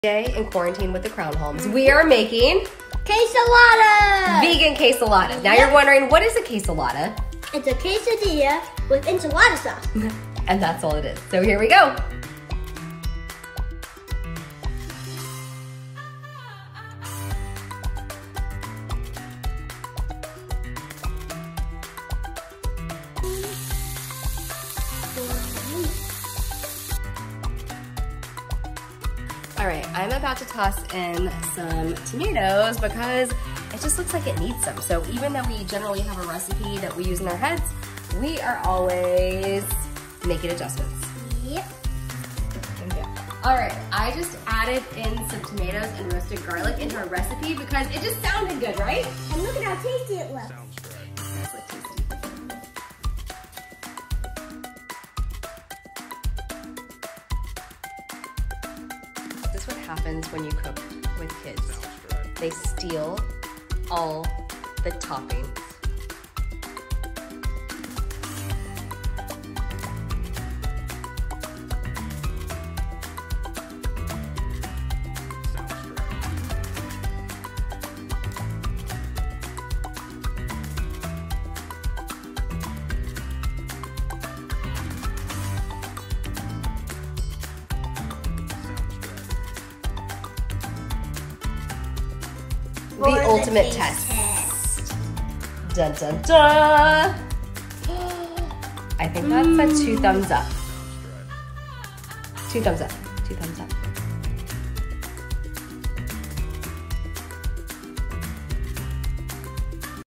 Ep. 3 Quarantine with the Crownholm’s: Making Vegan Quesaladas
On Ep.3 of Quarantine with the Crownholm’s we are making one of our go-to dinners of Vegan Quesaladas (quesadilla + enchiladas together)
Super easy and a kid fave! Love that you can take the leftover beefless beef filling and make into loaded vegan nachos the next day...so no waste! It’s such an easy way to get some extra spinach and mushrooms into their diets without them noticing Mom Win!